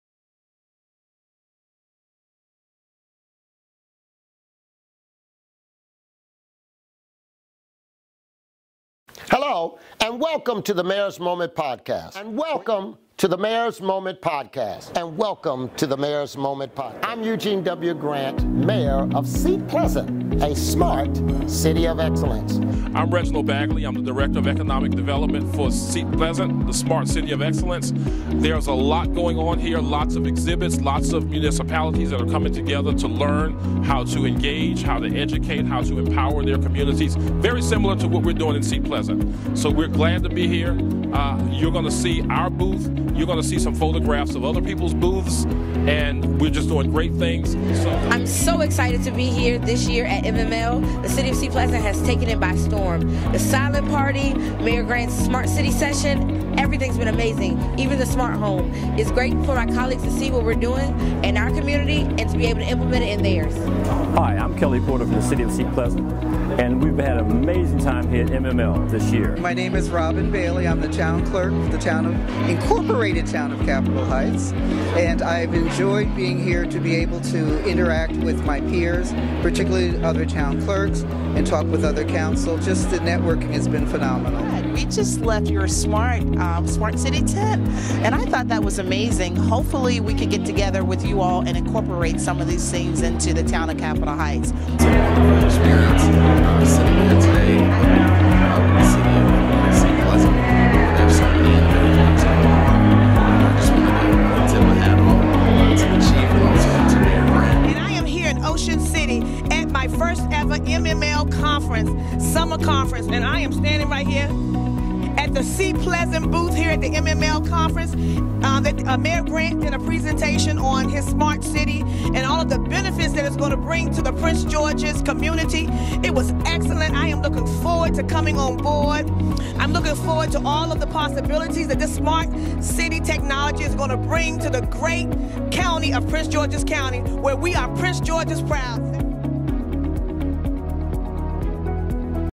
Mayors Moment Recap of 2019 MML Summer Conference
Today, we post a Recap of the 2019 Maryland Municipal League (MML) Summer Conference. This year’s conference was incredible.

Representatives and Conference Delegates from each of Maryland’s 157 municipalities were in attendance. We applaud the hard work of Scott Hancock, Executive Director of MML and his team for an incredible experience. Seat Pleasant was on fire. We had an awesome exhibit, great workshop and Silent Party. The conference allowed the City Council, Executive Team and a few Staff members attend workshops and classes that provided invaluable information. This video is a few highlights. Next week we will feature more from the conference. We encourage you to continue to follow our digital transformation as we are the Worlds First Authentic Small Smart City.